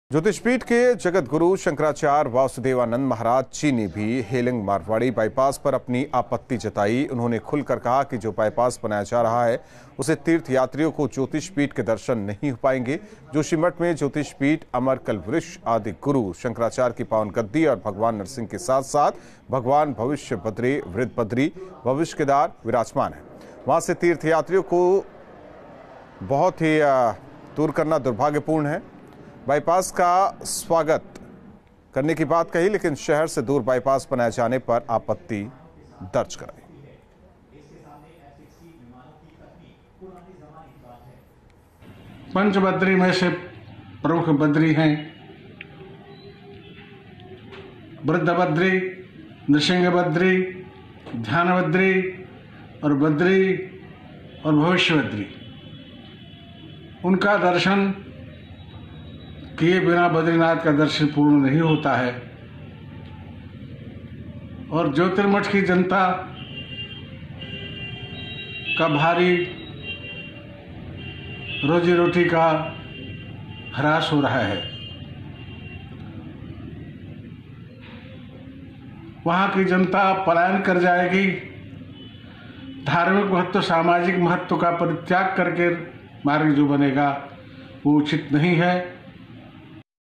ज्योतिष महापीठ ने हेलंग मारवाड़ी बाईपास पर जताई आपत्ति, केंद्र और राज्य सरकार से की शिकायत
उत्तराखंड: ज्योतिष महापीठ ने हेलंग मारवाड़ी बाईपास पर जताई आपत्ति, केंद्र और राज्य सरकार से की शिकायत.